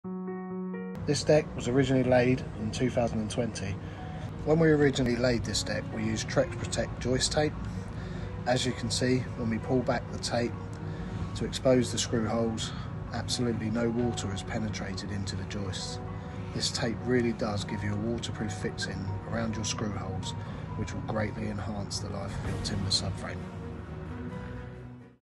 Trex Protect Joist, Beam and Rim Tape - US
Watch how Trex Protect Joist, Beam and Rim Tape protects the deck frame from moisture and keeps it dry years after installation.